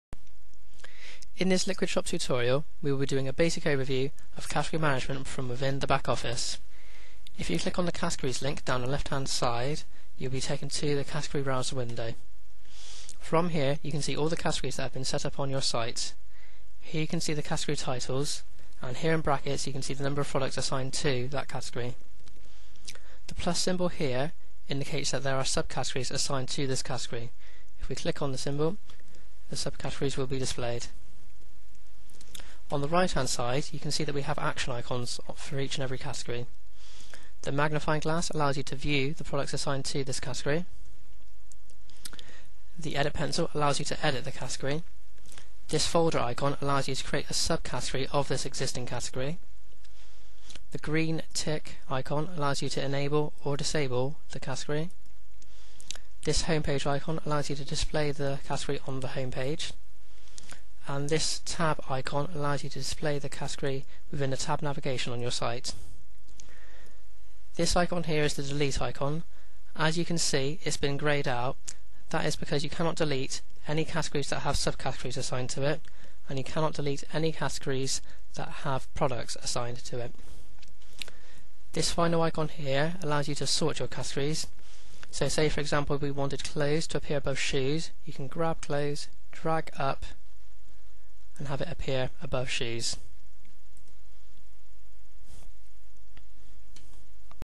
Basic Category Management Overview
A basic overview of category management within the LiquidShop backoffice.